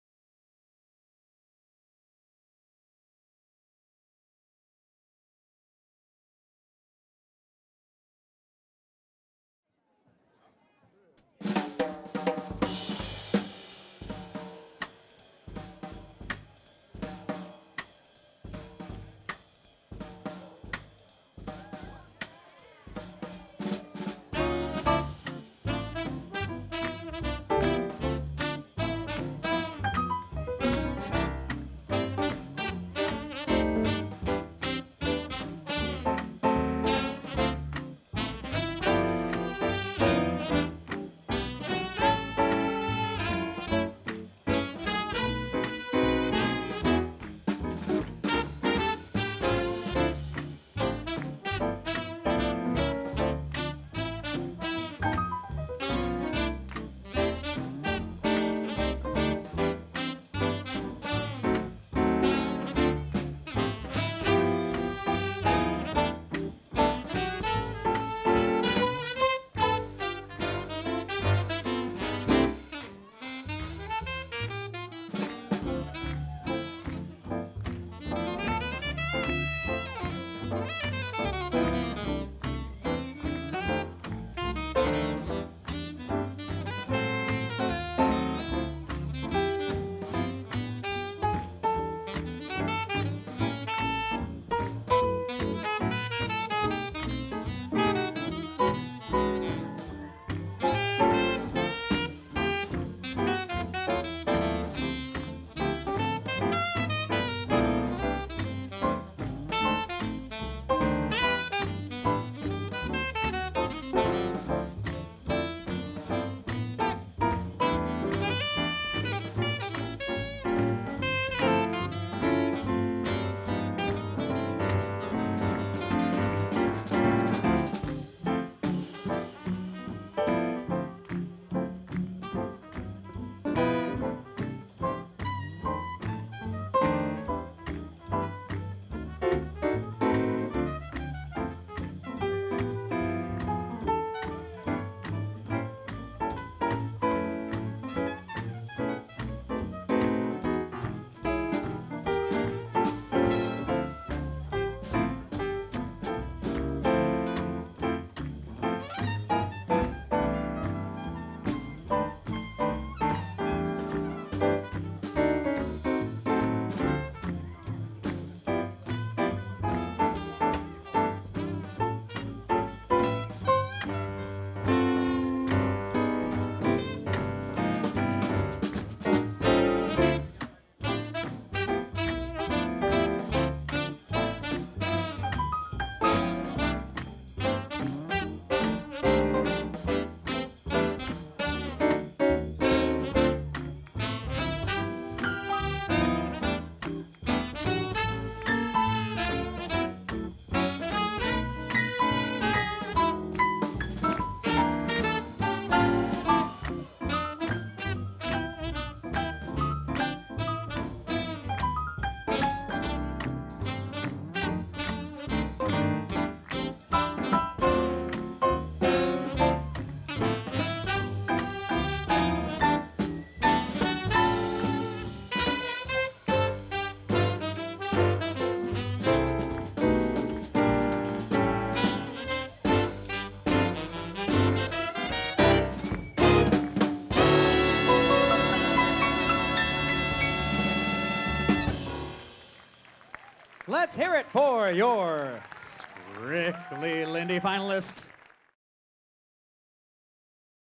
The Snowball 2015 - Strictly Lindy Finals - All Skate
The swing dance event of the year.
Stockholm, Sweden December 26 - January 1.